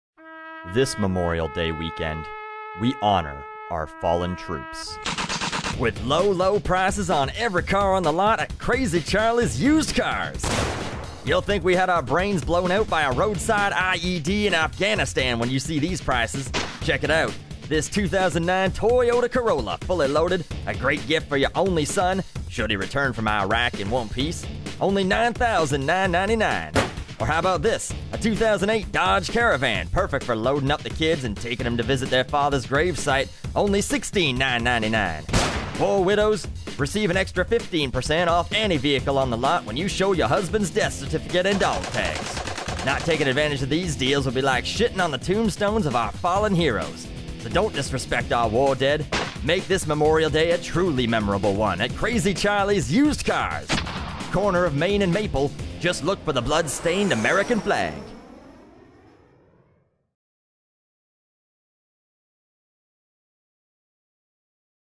Crazy Charlies Memorial Day Sale
Who says advertising needs to be tasteful to sell used cars? Not Crazy Charlies! This Memorial Day, come on down and save, save, SAVE!

More parodies and all that fun stuff at A Dose of Buckley